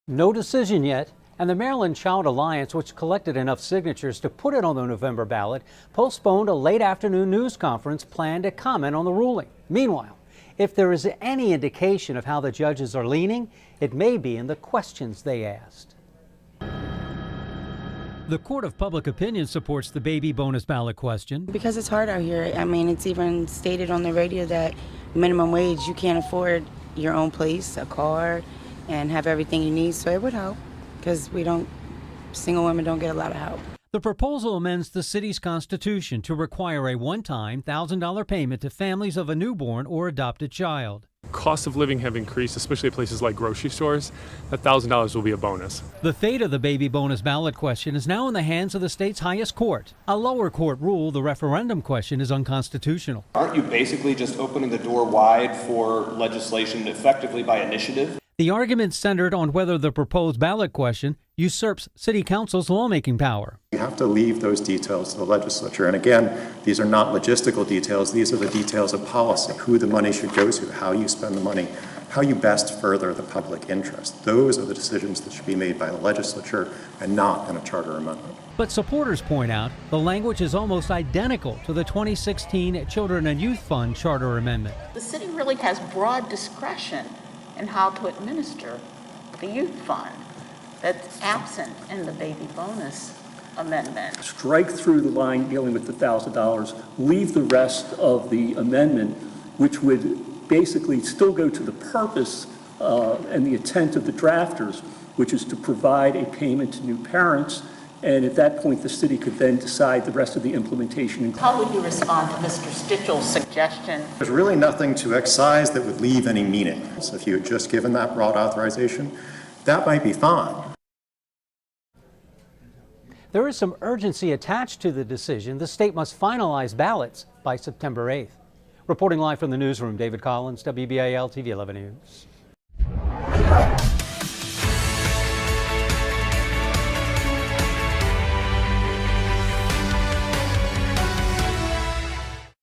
Baby bonus question reaches state supreme court
The fate of the controversial baby bonus ballot question is now in the hands of the state supreme court. The justices heard arguments Wednesday on whether the proposed charter amendment is constitutional. The initiative provides a one-time, $1,000 payment to the parents of newborns or adoption of a child.